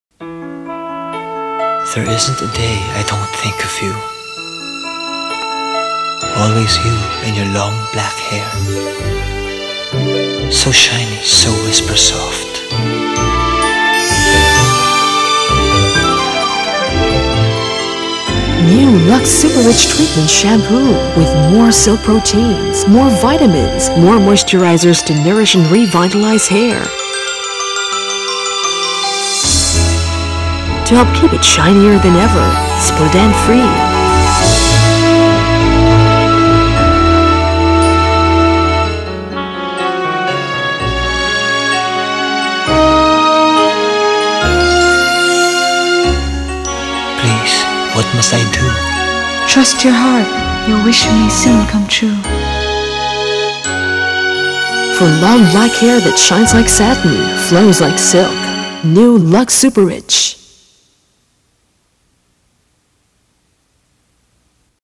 Lux Super Rich Treatment Shampoo "Afternoon" 60sec (Philippines, 1994)
With Lucy Torres-Gomez & Richard Gomez.

For long black hair that shines like satin. flow's like silk.

(c) 1994 Unilever Philippines PRC TOTAL QUALITY Paglilingkod namin sa inyo